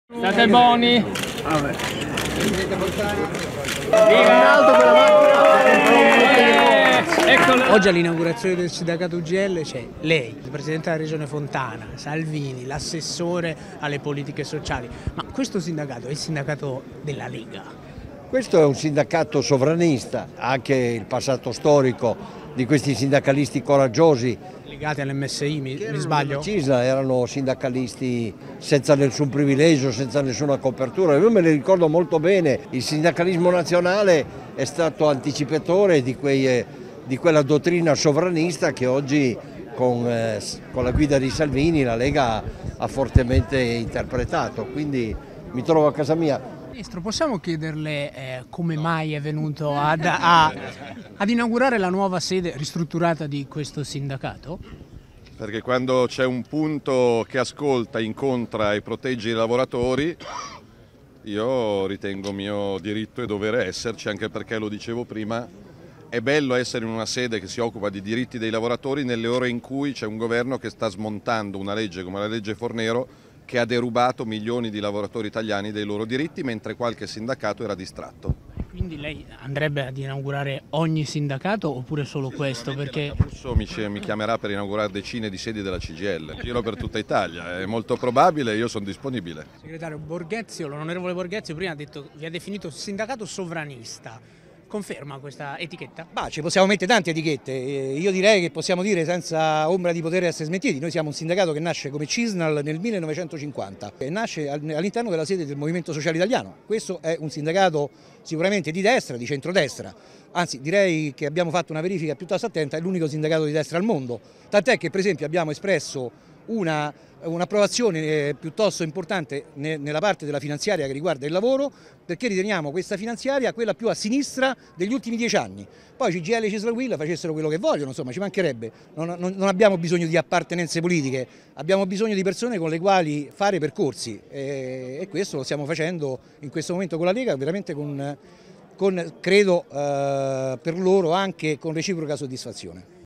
Salvini inaugura sede Ugl, il sindacato 'sovranista'. E ironizza:"Camusso mi chiamerà per sedi Cgil"
"Contro la legge Fornero", "di destra" e "di ispirazione nazionale". Nata dalle ceneri della Cisnal, che fu diretta espressione sindacale del Movimento Sociale Italiano, l'Unione Generale del Lavoro ha inaugurato a Milano la sua sede ristrutturata. Un'inaugurazione tutta leghista, visto che oltre al ministro dell'Interno Matteo Salvini, erano presenti anche Mauro Borghezio, il governatore lombardo Attilio Fontana e il sottosegretario Claudio Durigon, "Qui mi sento a casa - ha detto Borghezio - perché il sindacalismo nazionale è l'anticipatore della nostra dottrina sovranista". "Gli altri sindacati erano distratti sulla legge Fornero" ha precisato Salvini che poi ha fatto ironia sul fatto che "Camusso mi chiamerà e dubito che la Camusso mi chimaerà per inaugurare decine di sedi Cgil". .  .di Andrea Lattanzi